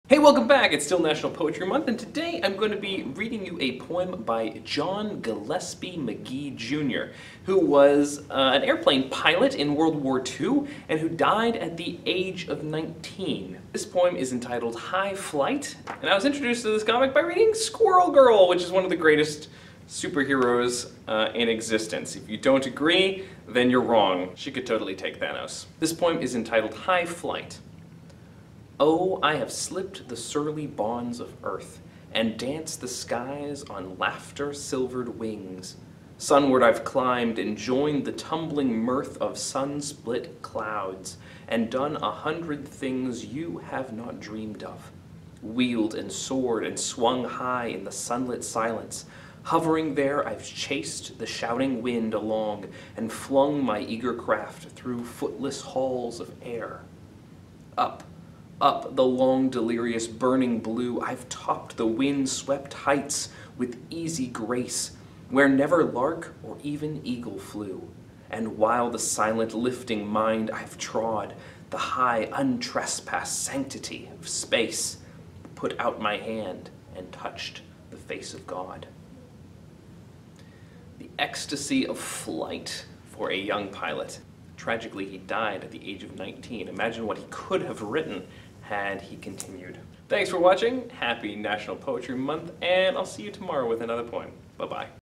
"High Flight" by John Gillespie Magee, Jr. NATIONAL POETRY MONTH DAY 20!
I read a poem by a WWII pilot that I learned about from a Marvel comic!